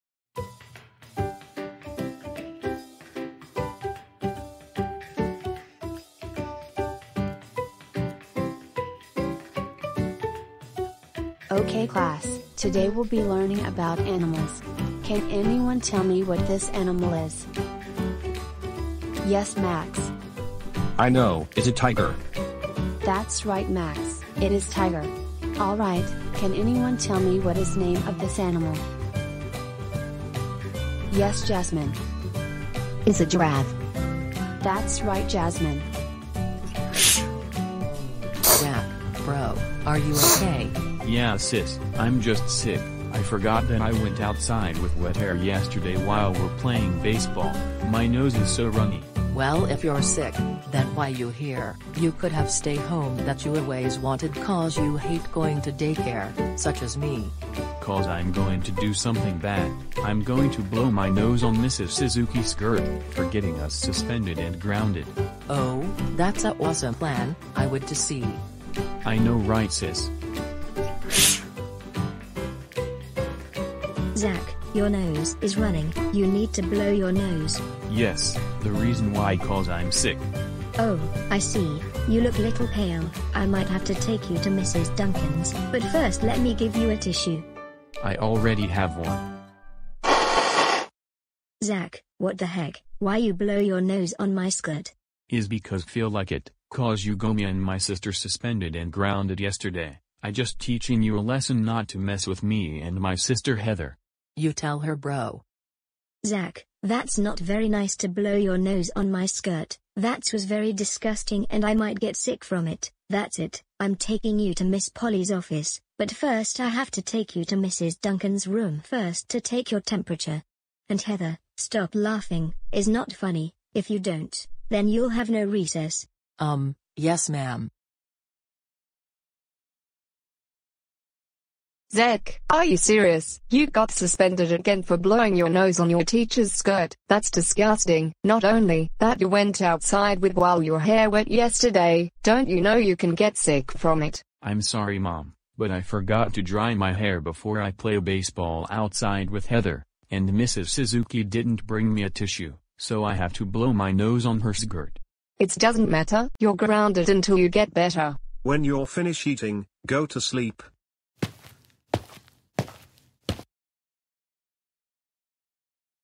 Zack Blow His Nose On Mrs Suzuki Skirt/Grounded
Zack did that for revenge after yesterday they got suspended for insulting and gave Mrs Suzuki a rotten apple. Zack is nasty child